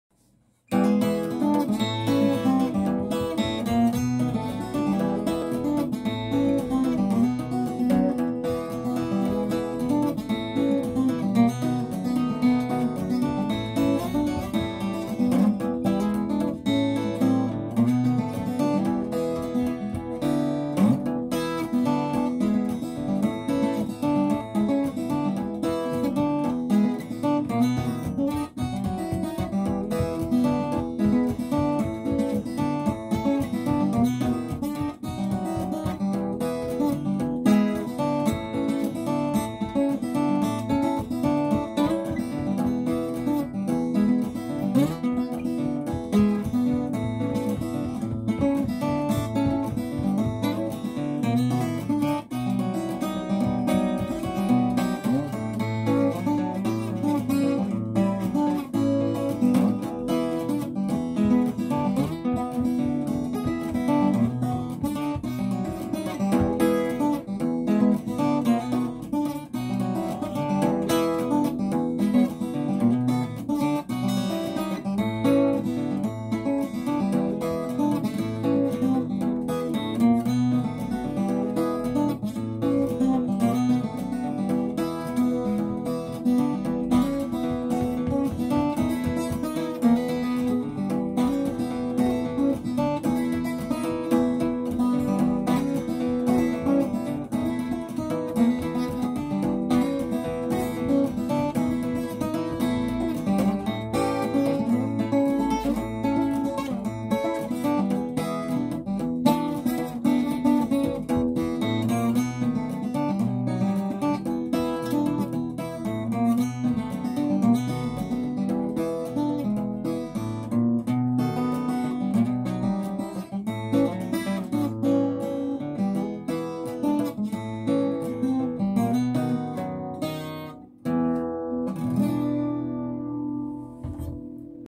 12 String Rockabilly in E!
A 3 Chord Exploration With A Few Inversions Thrown in To Add Some Tonal Variation!